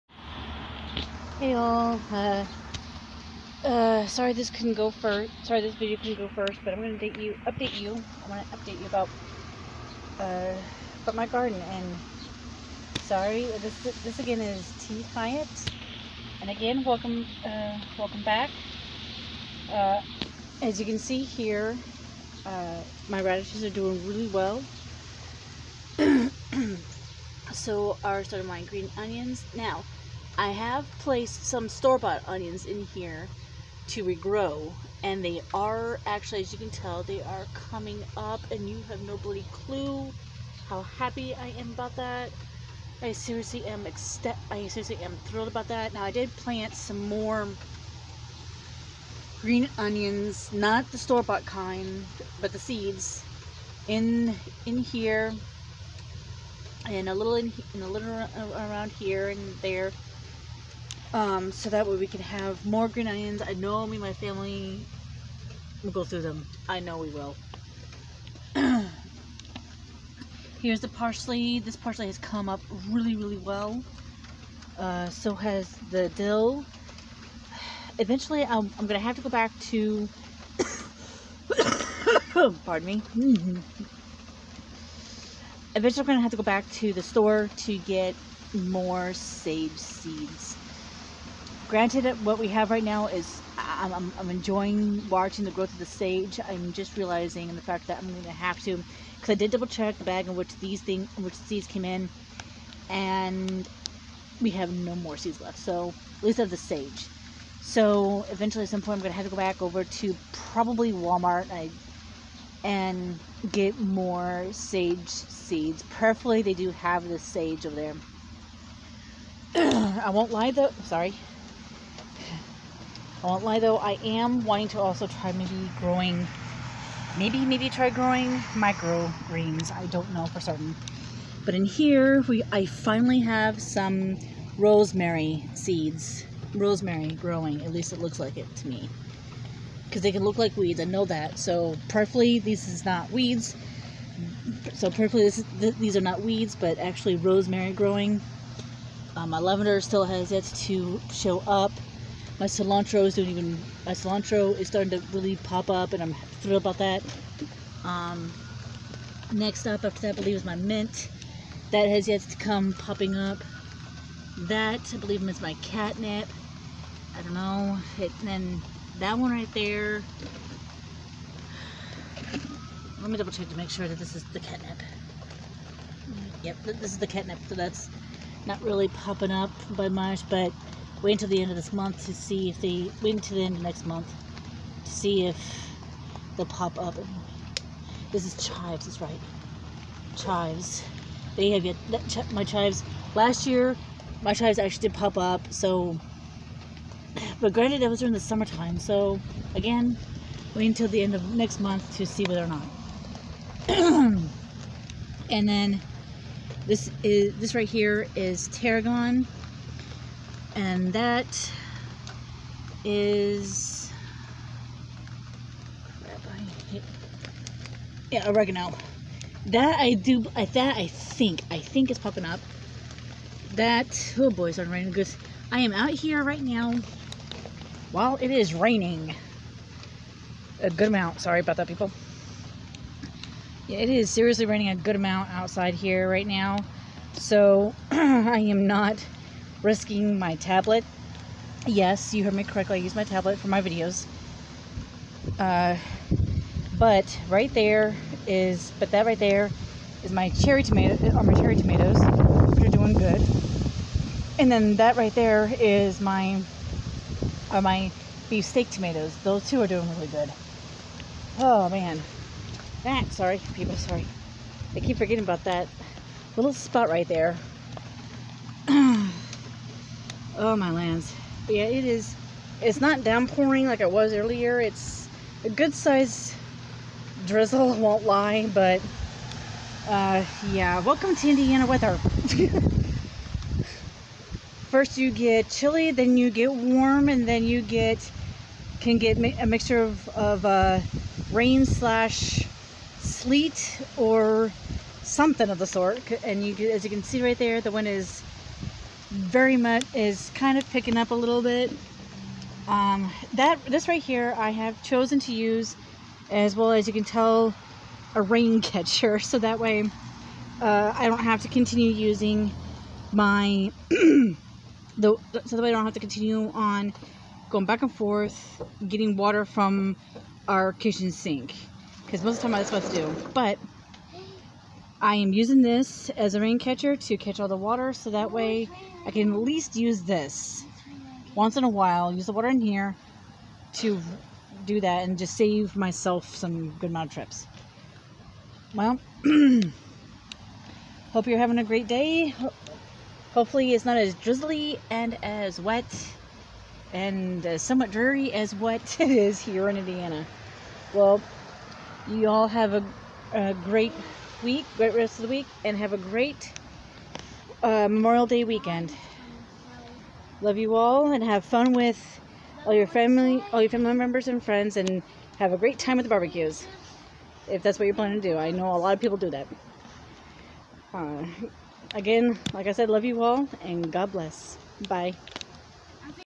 a short look at my garden 🙂
Looking at the progress of the garden.   share your thoughts! 🙂